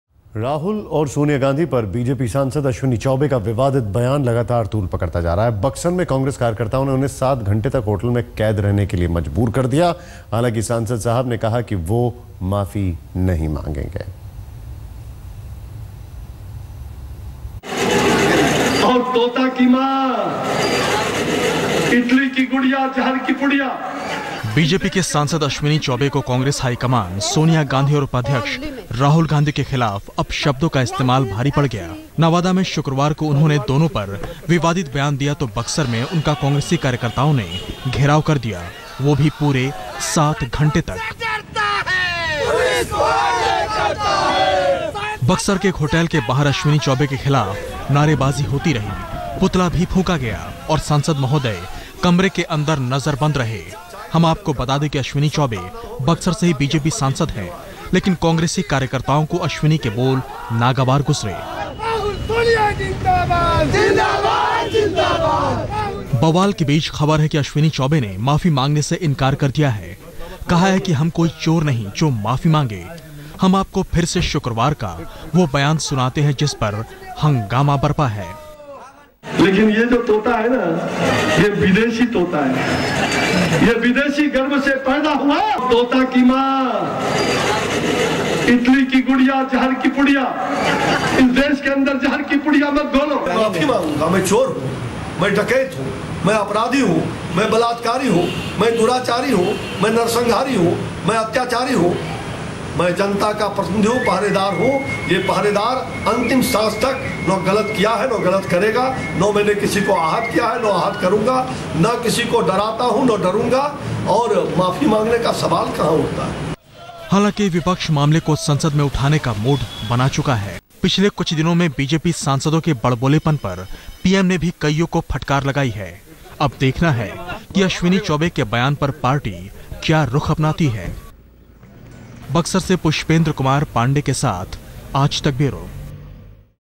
India 360: Protests Against BJP's Chaubey For Anti-Rahul, Sonia Comments
Protests continued for close to seven hours in Buxar against BJP's Ashwini Chaubey for comments against Rahul, Sonia Gandhi.